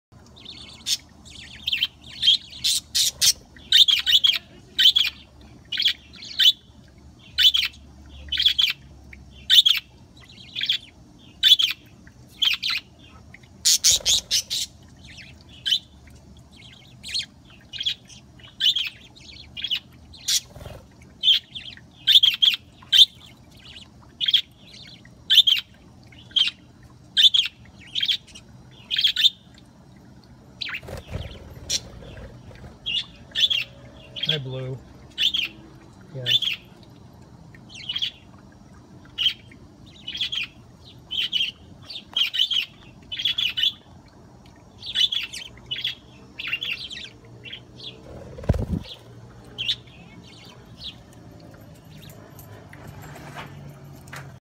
Baby Budgies in the Aviary (First Time) - 3
Daddy Blue has been flying out of the cage when we open his door. He flies and chirps and the babies respond to him. I had a theory that maybe he wants to work to teach them how to fly, thus giving them some aviary time. This video kind of shows that as he flies between the stationary perch, the perch tree, and my shoulder.